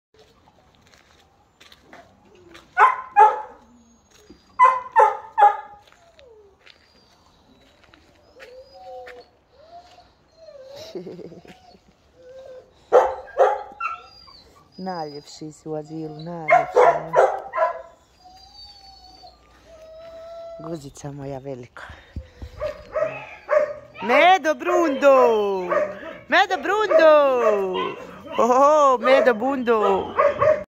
Neues von unserem Medo #2
Noch ein kurzes Video, dass unseren Medo zeigt. Medo ist ein Tornjak Rüde, der es liebt zu kuscheln und sich gut mit anderen Hunden versteht. Leider sucht er immer noch ein Zuhause.

Dein Hands4Animals-Team